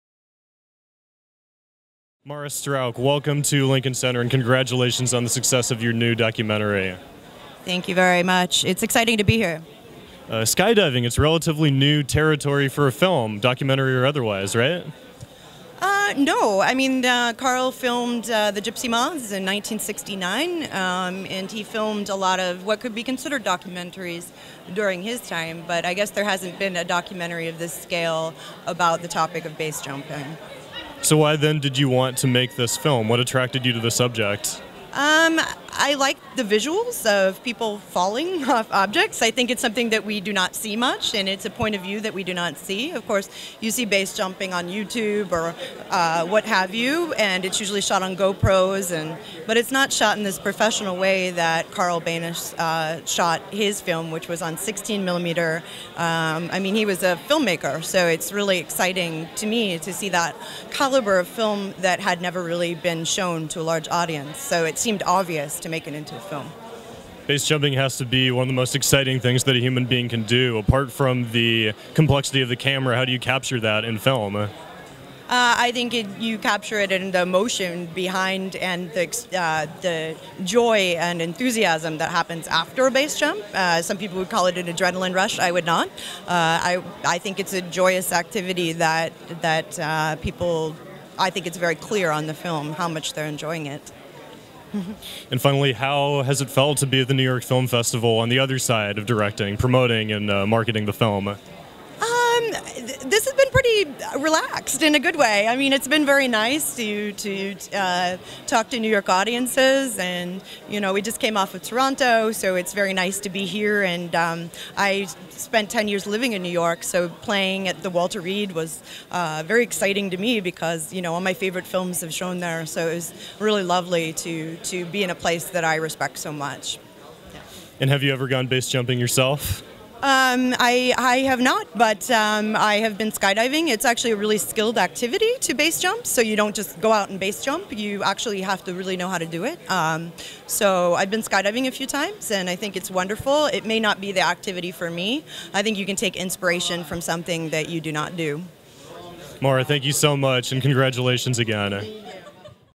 NYFF52 "Sunshine Superman" Red Carpet | Marah Strauch
Filmmaker Marah Strauch talks about her film "Sunshine Superman" before its U.S. premiere at the 52nd New York Film Festival.

Marah Strauch’s evocative movie, which tells the incredible story of Carl Boenish, the exuberant inventor of BASE jumping (parachuting from a fixed object), is in the end a film about joy and freedom.